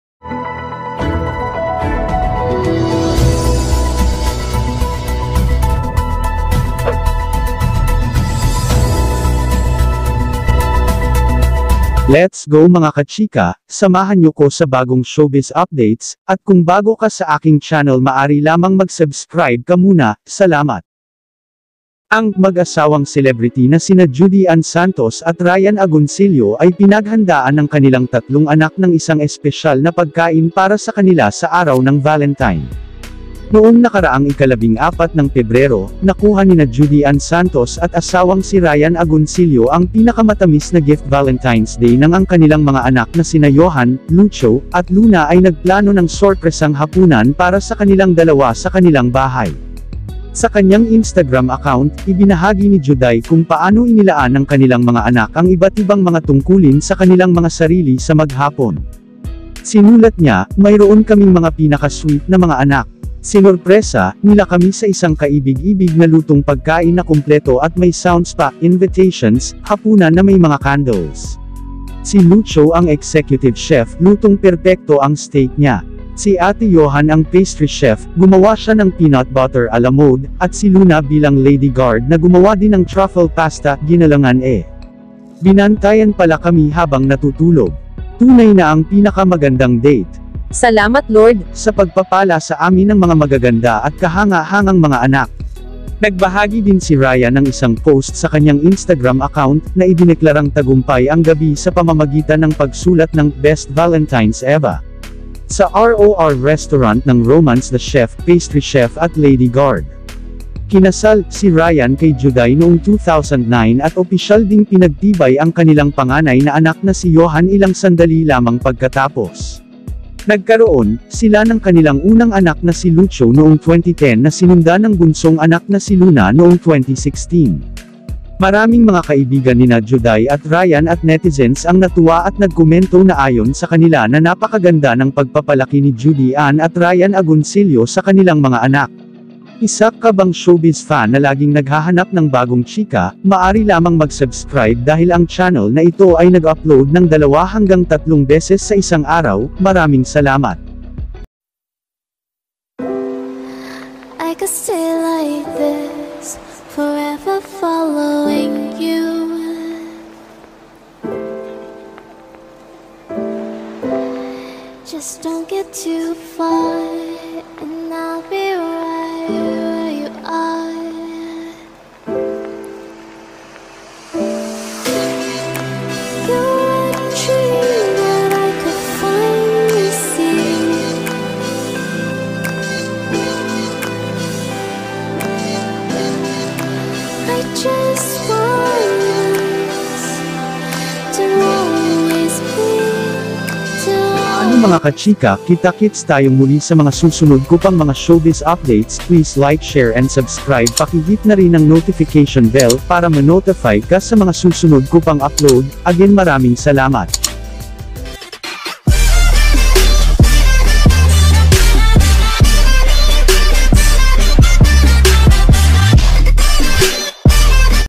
GANITO pala Ang VALENTINE'S DATE ni JUDY ANN SANTOS & RYAN AGONCILLO D'Best TALAGA!!!
The best Valentine's date Nina Judy Ann at Ryan Agoncillo.